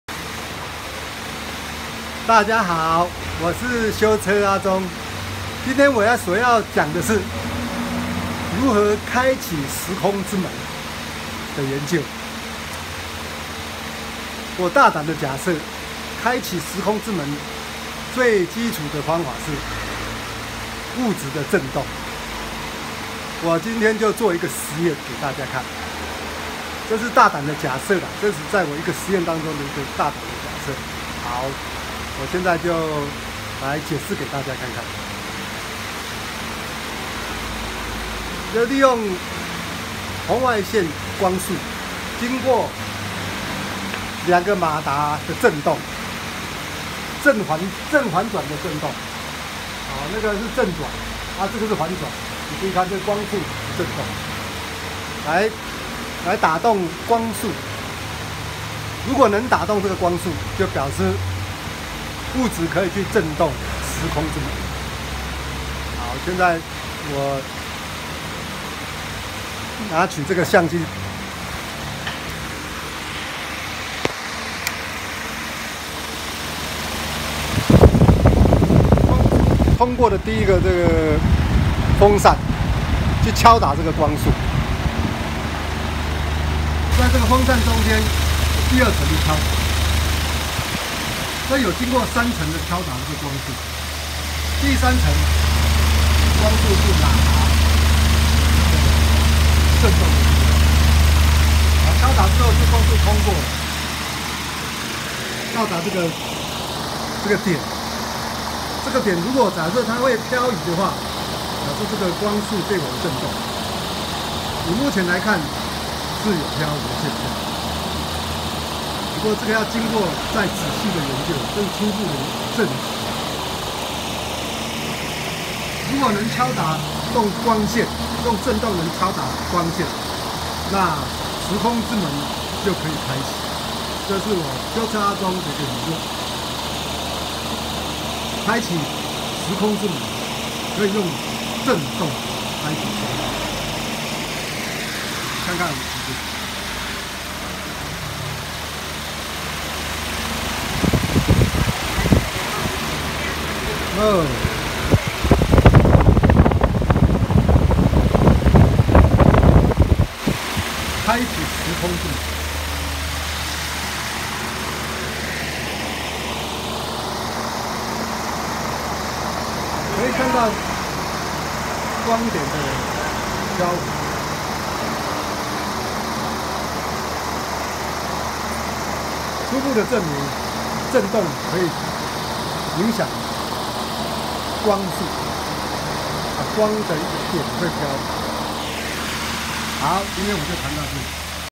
可以人造重力場嗎？實驗 1
阿忠大膽的理論
用物質的轉動可以製造人造重力場當轉動加上振動的力量，打破宇宙平衡之力量時，力塲切割，時空之門可以開啓，
光束射入三層振動，將光束飃移，初步証明振動可以彎曲光線，阿忠初步實驗。